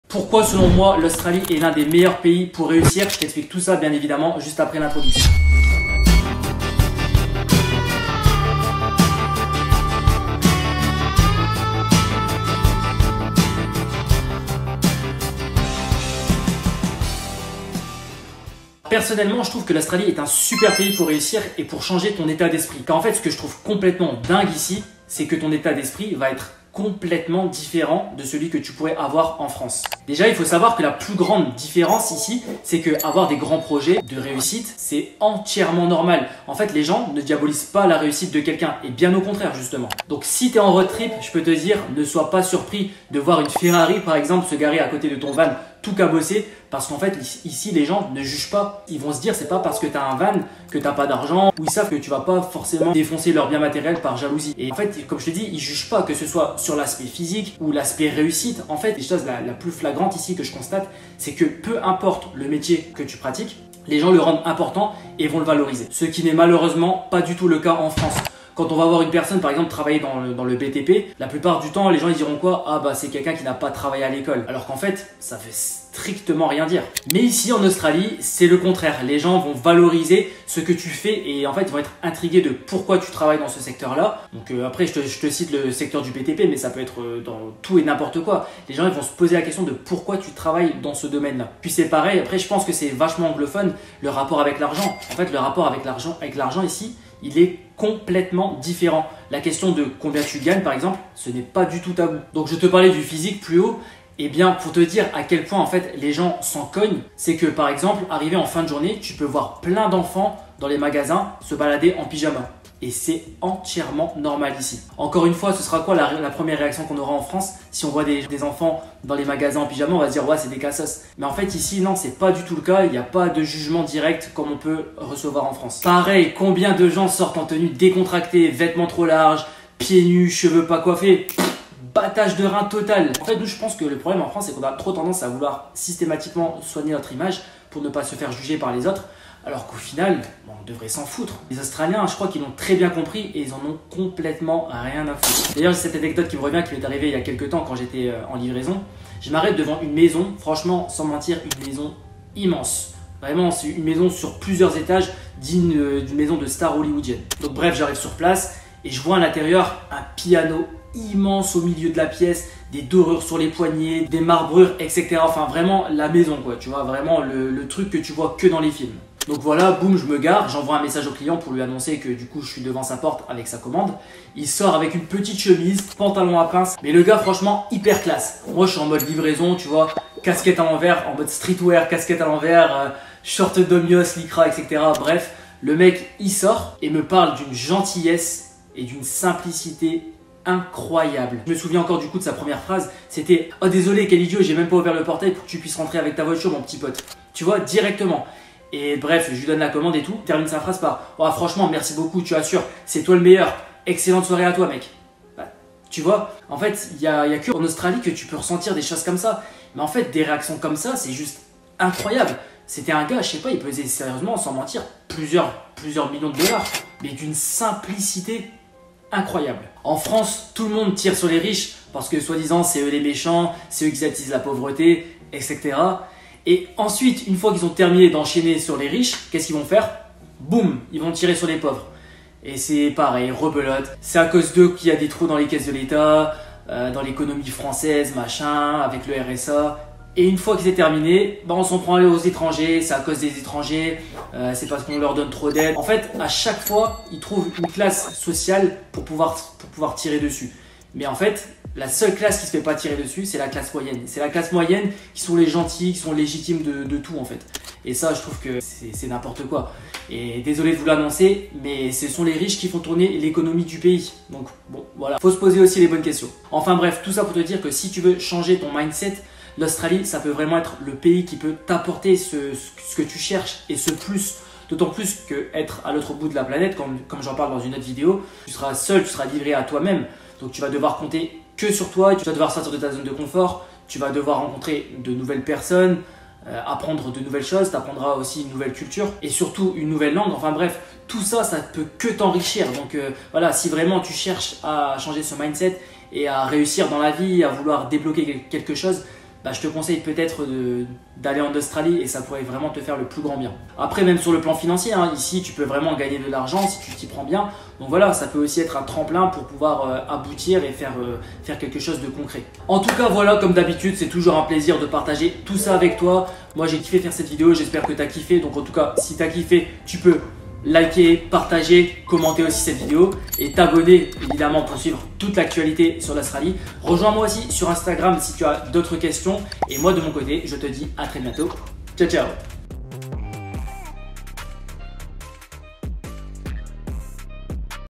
L’AUSTRALIE VA TE RENDRE RICHE !
L'Australie offre un environnement propice à la réussite et au changement d'état d'esprit.

Contrairement à la France, les grands projets y sont encouragés et la réussite n'est pas diabolisée.
 Les Australiens ne portent pas de jugement sur les apparences ou le succès financier. Chaque métier est valorisé, et les questions d'argent ne sont pas taboues. L'importance de l'image personnelle est moindre, permettant une plus grande liberté d'expression.

Les rencontres avec des personnes simples malgré leur richesse sont courantes, ce qui contraste avec la stigmatisation des riches en France. L'Australie offre ainsi une opportunité de changement de mentalité, grâce à son éloignement géographique et la nécessité de sortir de sa zone de confort. C'est un endroit où l'on peut s'enrichir intellectuellement et financièrement, à condition de s'investir.
Et c'est ce que je veux t'expliquer aujourd'hui à travers cette vidéo